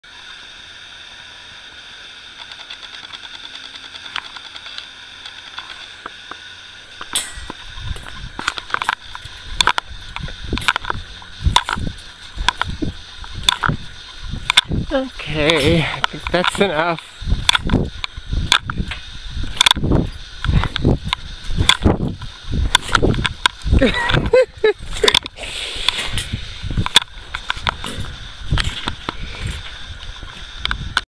Me on trampoline 08 10 2012 Daniel
Having fun today at work. Working safe trampoline at one of the properties. I couldn't resist.